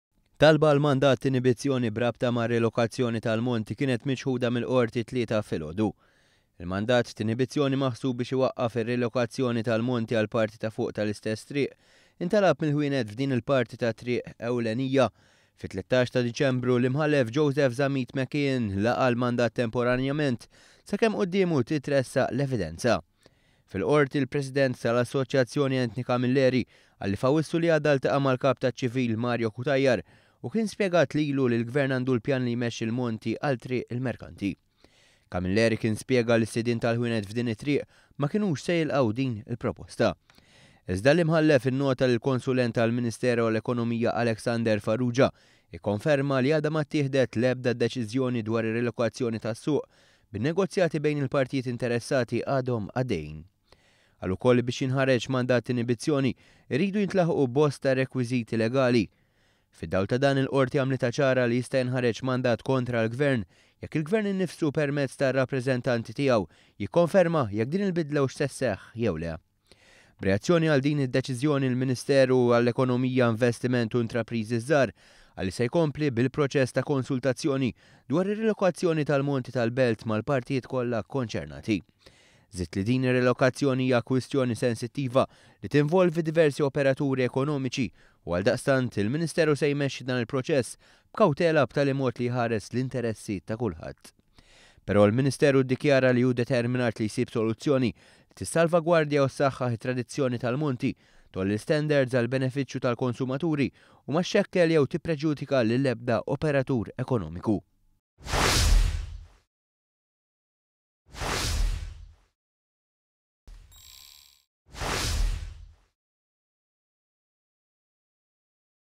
IL-QORTI MA TILQAX MANDAT DWAR IL-MONTI.
ONE.COM.MT

Talba ta’ sidien ta’  ħwienet minn Triq il-Merkanti biex il-Qorti twaqqaf il-possibbilta' li l-monti jitla għal parti ta’ fuq tal-istess triq ma ntlaqgħetx.